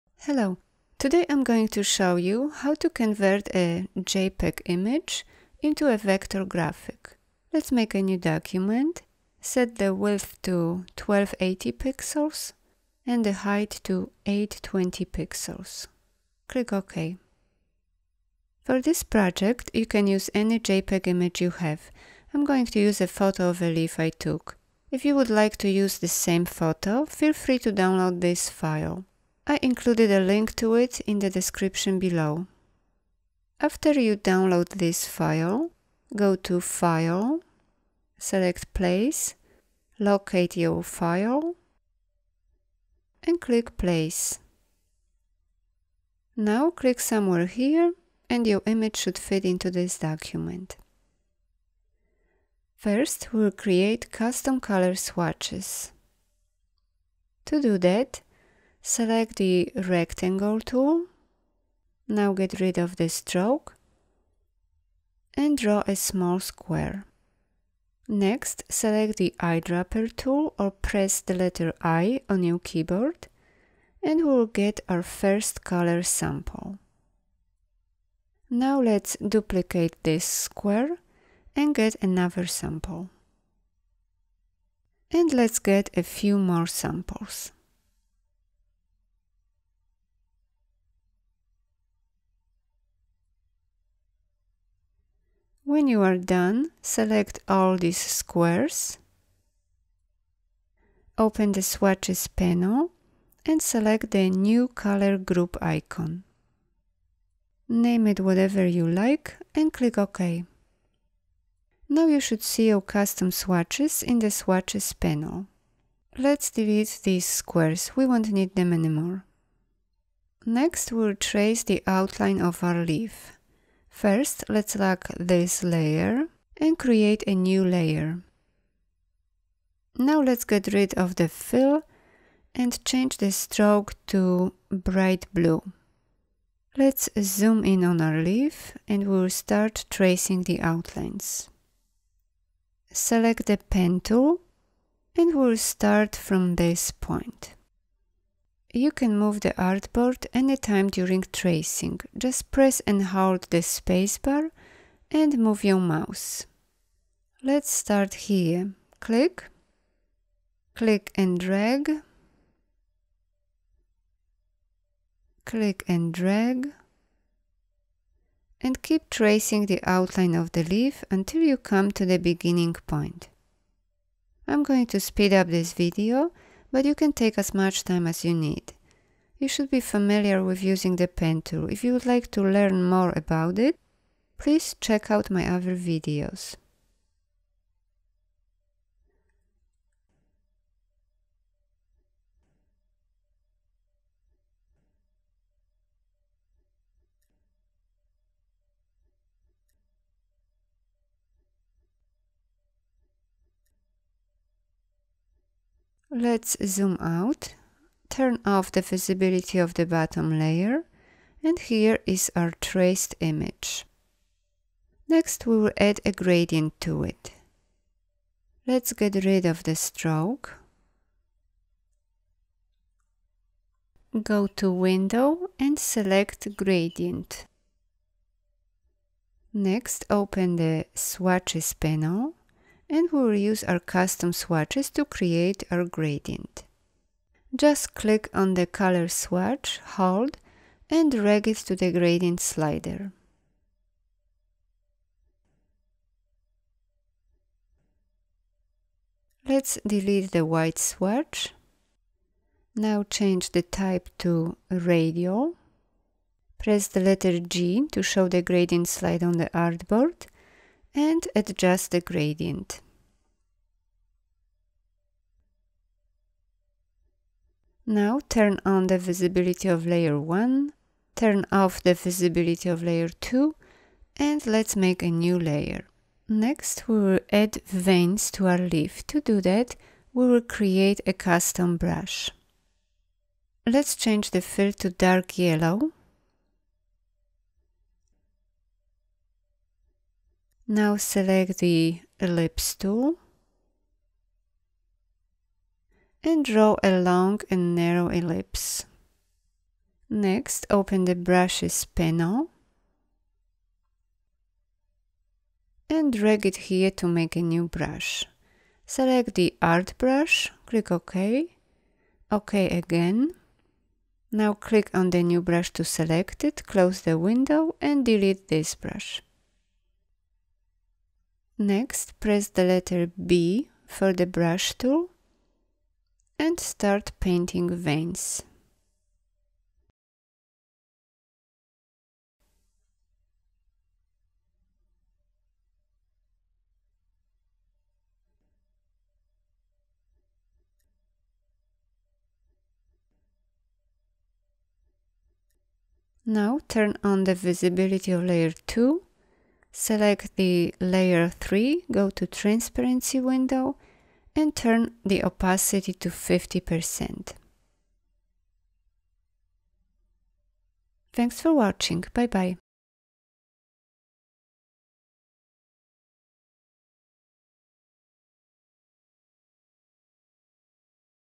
How to Convert a JPEG Image Into a Vector Graphic in Adobe Illustrator
Easy way to convert your photograph into a vector graphic using the pen, gradient and eyedropper tools.
Here is a link to my JPEG image I used in this video
TUTORIALS